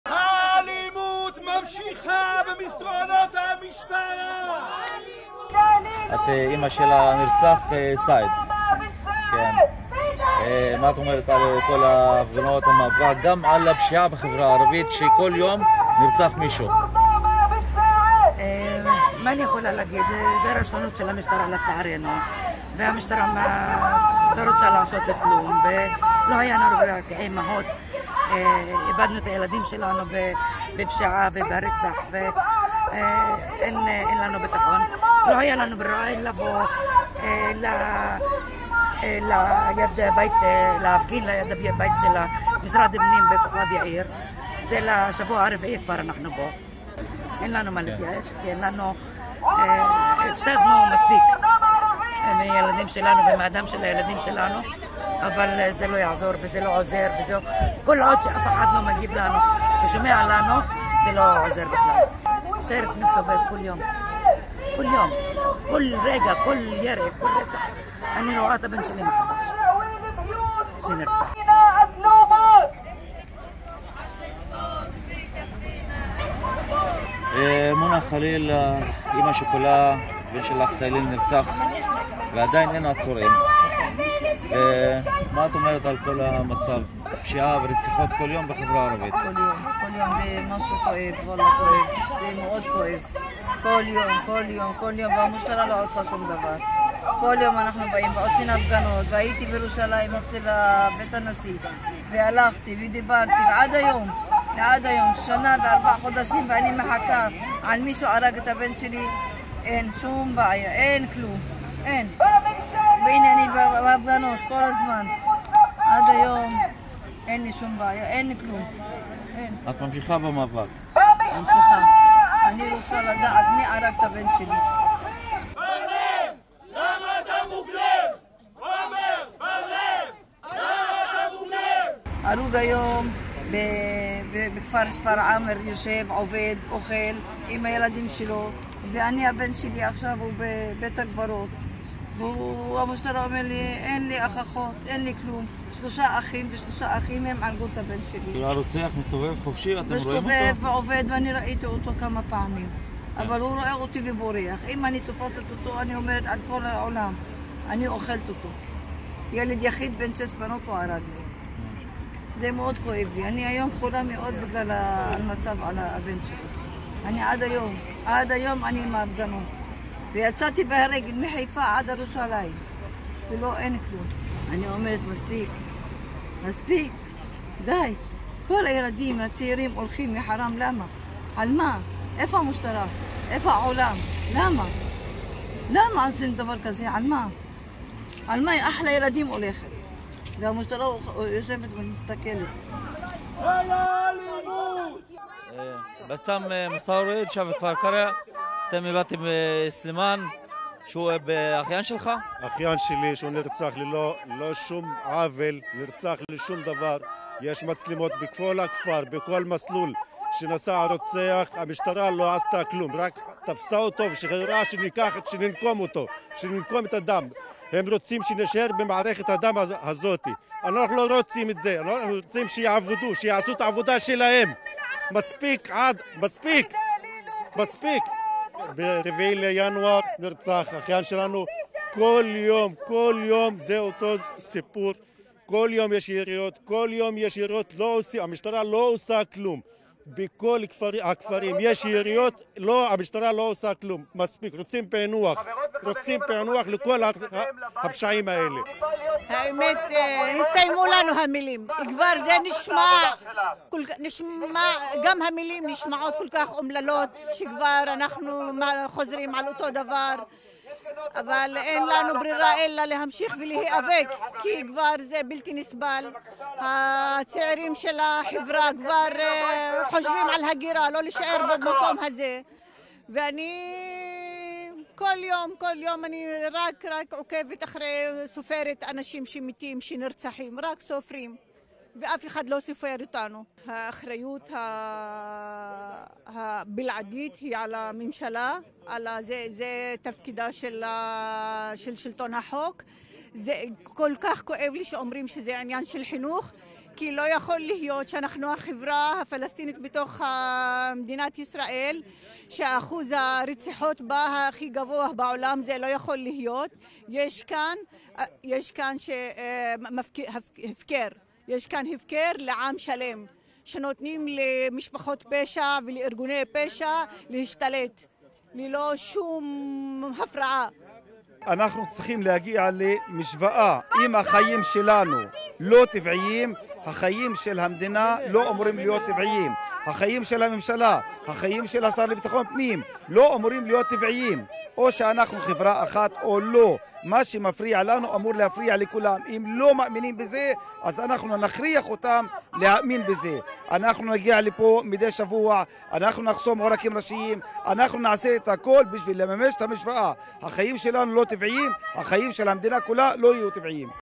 הפגנה על האלימות במגזר הערבי
אחרי שני מקרי רצח נוספים בחברה הערבית בסוף השבוע, עשרות התכנסו בכוכב יאיר כדי למחות על הטיפול בפשיעה. "המשטרה אמרה לי שאין הוכחות, אבל אני יודעת מי הרג את הבן שלי. כשהוא רואה אותי הוא בורח", אמרה מונה חליל, שאיבדה את בנה. מספר הנרצחים מתחילת השנה כבר הגיע ל-97